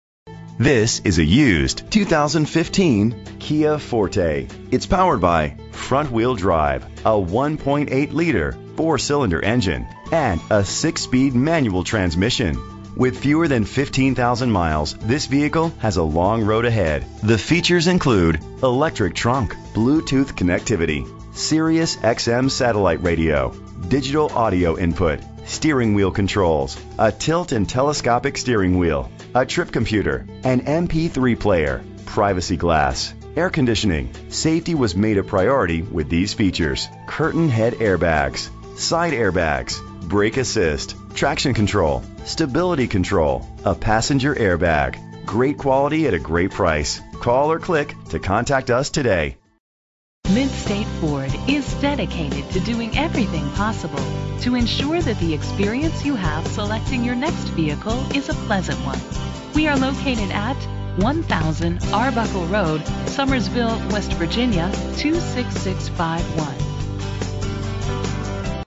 2015 Kia Forte - Summersville WV
Phone:
Year: 2015
Make: Kia
Model: Forte
Trim: LX
Engine: 1.8 liter inline 4 cylinder 16 valve
Transmission: 6-Speed Manual
Color: Aurora Black
Mileage: 10284
Address:
1000 Arbuckle Road